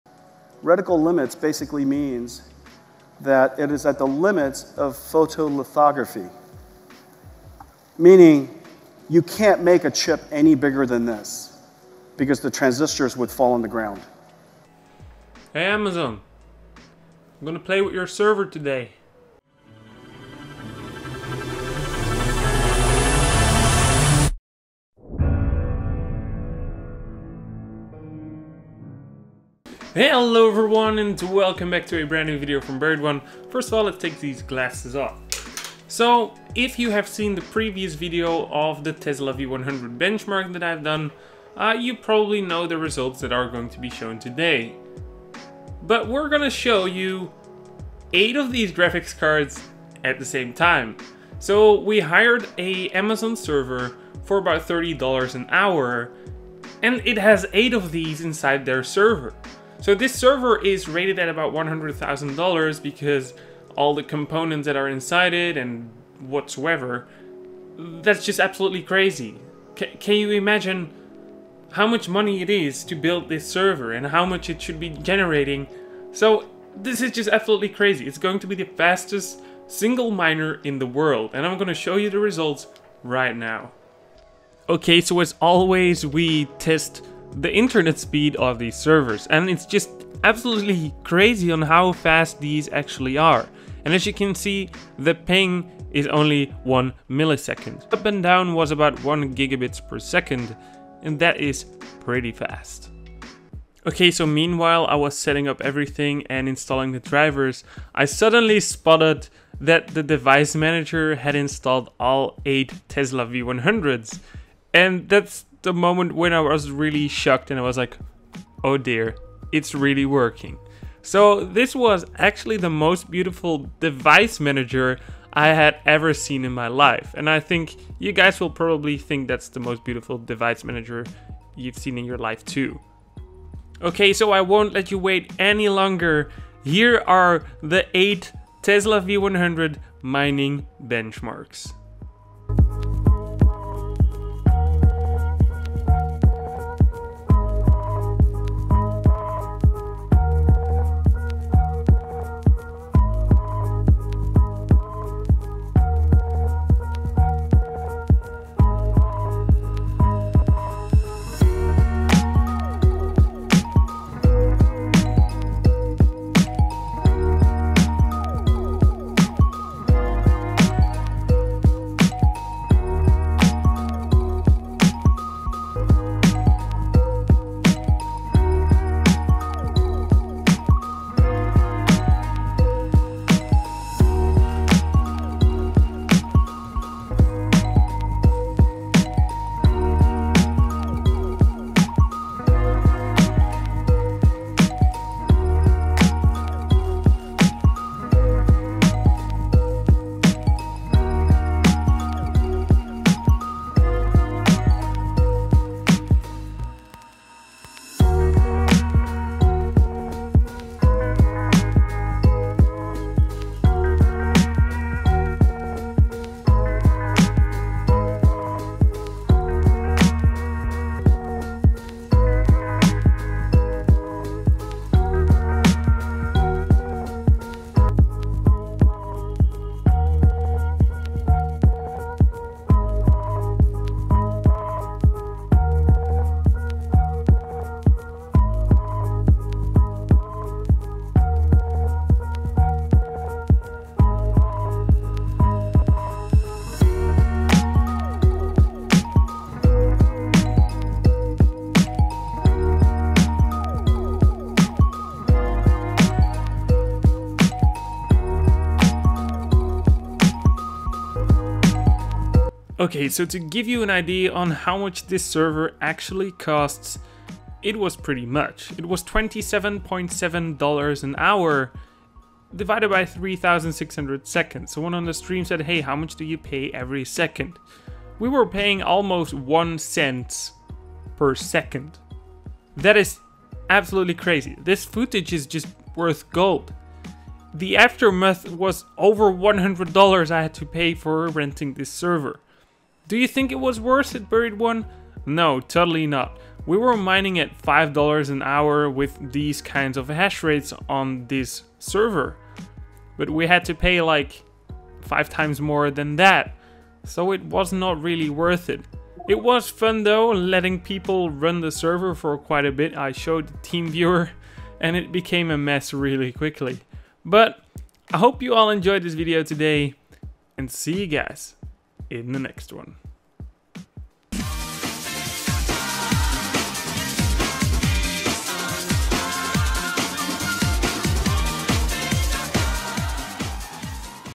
8X Tesla V100 WORLD RECORD, Fastest Miner RESULTS! On a $100.000 AWS Server.
Today i show you the Mining hashrates of a $100.000 Server from Amazon with 8X NVidia NVlink Tesla V100 GPU's on board.
The result is the worlds fastest miner ever seen on a single system.
The actual benchmarks were performed on the livestream we did a few days ago, the quality of the video is not the best but it should be easy to spot the record numbers it reached anyway!